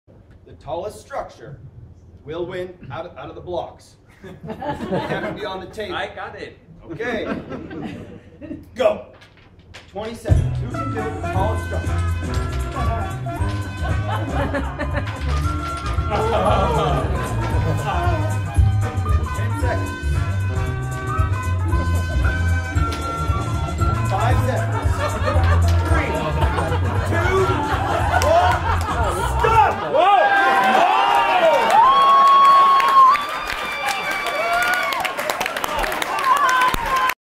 IMPROMANIA! Professor Spontaneous Impro-Show mit mit Lee White u.a. - Jenga
Urban Luig (Paternoster, Berlin) und Konstanze Kromer (Theatersport Berlin) spielen Jenga im Rahmen der IMPROMANIA! 2013 (Impro-Festival von Theatersport Berlin) darum, wer sich gleich nasse Schwämme in die Hose stecken darf.